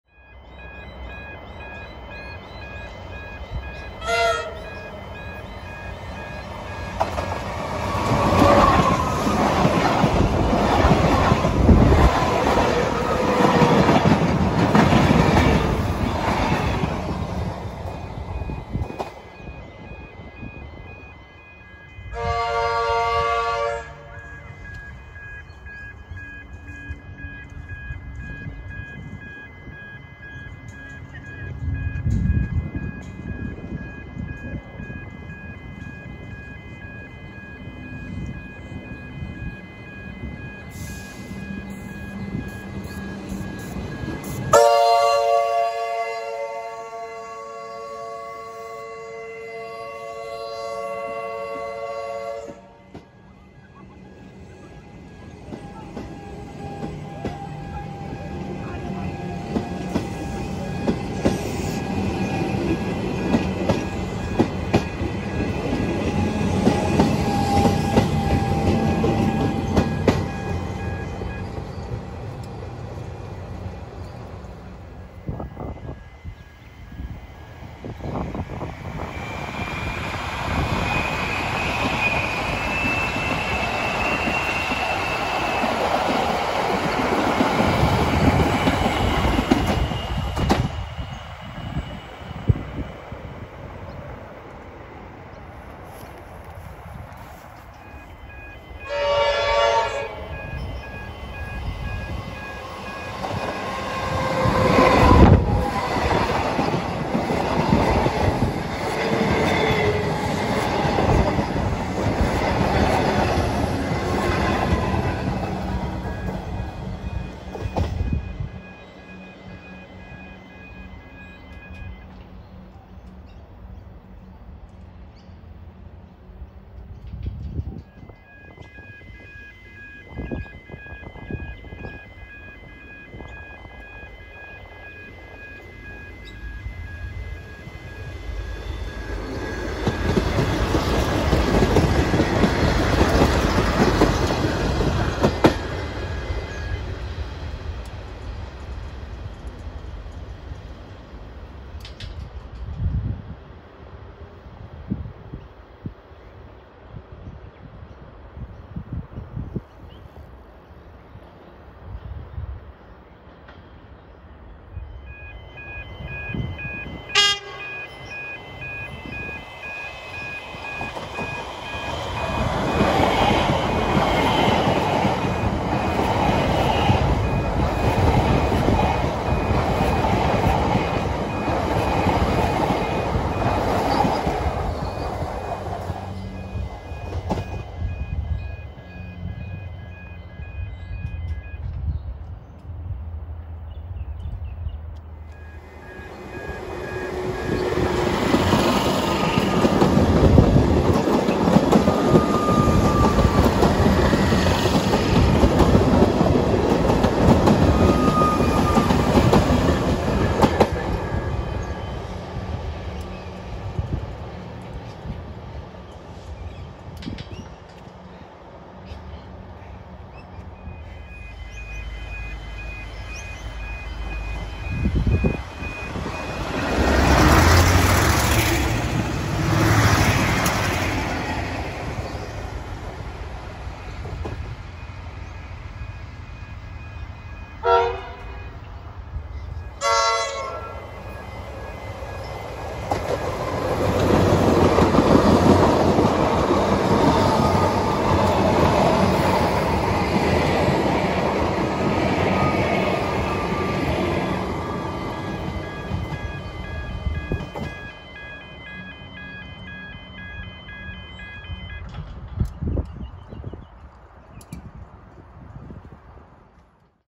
Trackside in Pascoe Vale
We see here some Comeng and Siemens Trains as well as two V/Line Sprinters during peak hour at a pedestrian crossing near Pascoe Vale station on the Craigieburn line. One Comeng was unfortunately slowed by a group of teenagers crossing the tracks

Location: Burgundy Street, Pascoe Vale, Victoria
Date: 20/1/2022